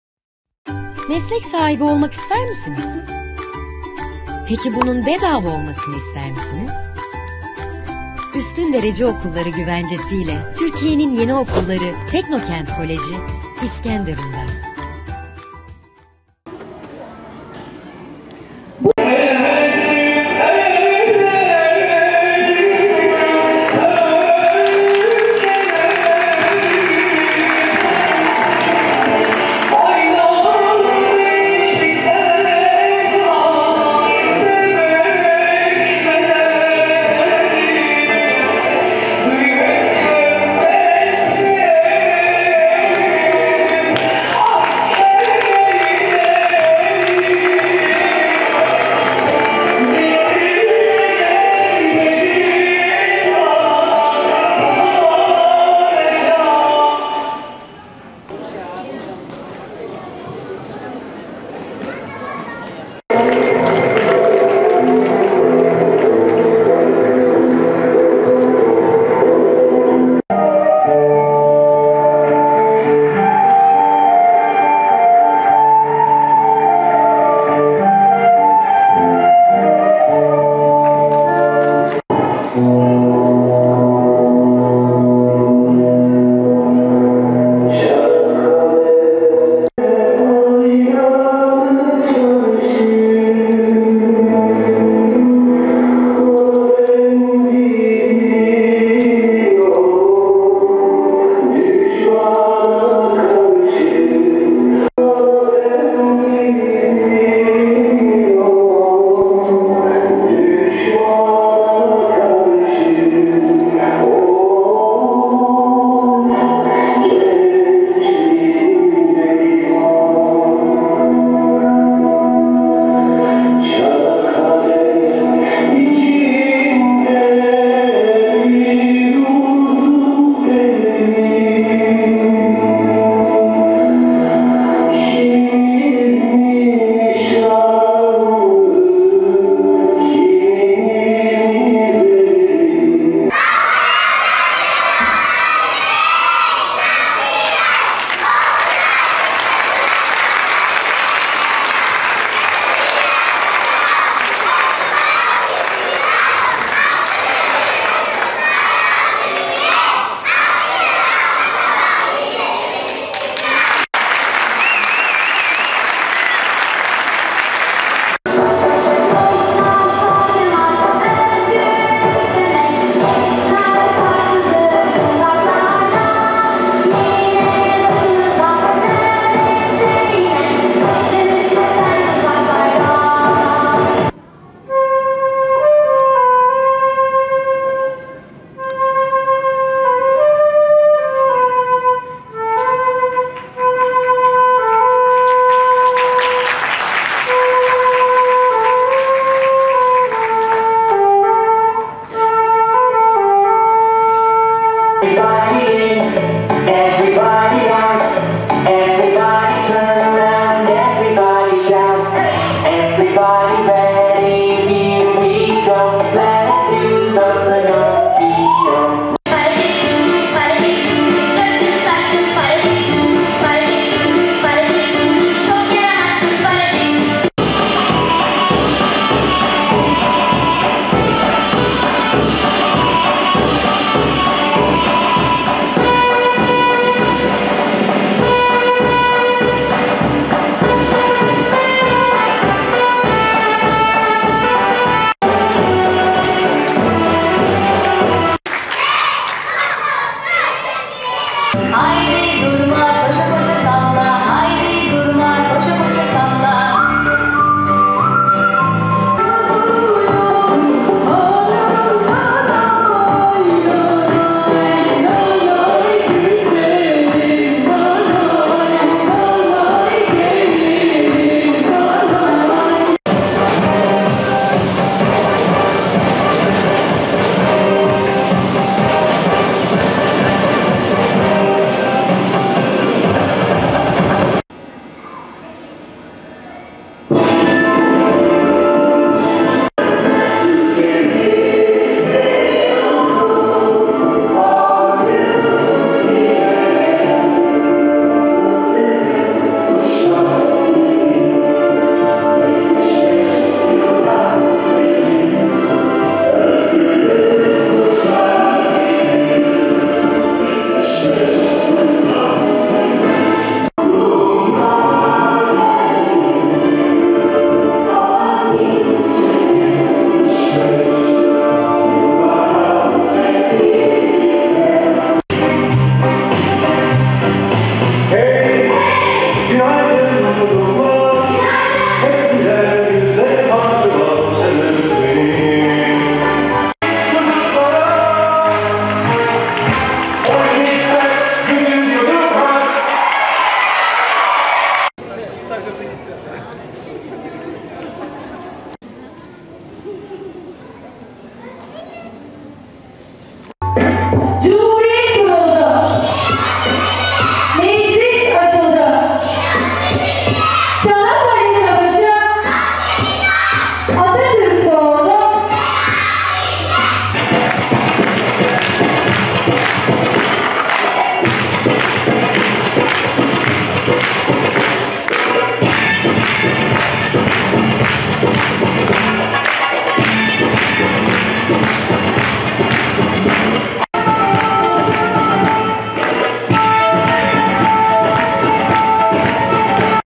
Çilek Çocuk Akademisinden renkli yıl sonu etkinliği…
Merkezi İskenderun'da bulunan Çilek Çocuk Akademisi okul öncesi eğitim kurumunda eğitim gören çocuklar düzenlenen yıl sonu etkinliğinde renkli gösterileriyle ilgi gördü.